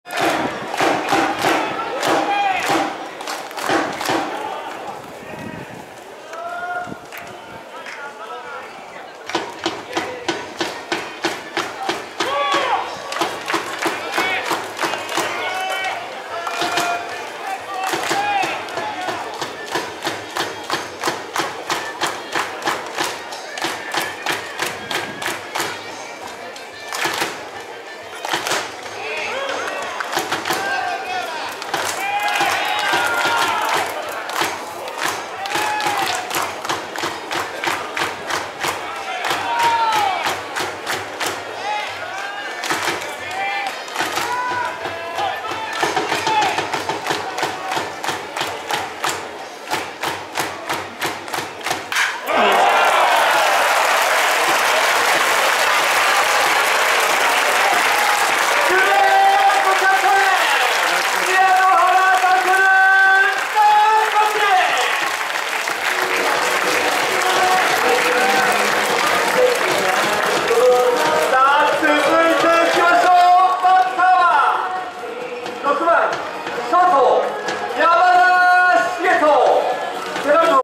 2021/5/29 【巨人3軍戦、決勝タイムリー】 宮之原 健 （埼玉武蔵ヒートベアーズ）
宮之原健　（東京・日大三高校 ⇒ 東京学芸大学 ⇒ BC福島 16~17 ⇒ 埼玉武蔵ヒートベアーズ 18~）　178cm80kg　両左　27歳

投手・笠島尚樹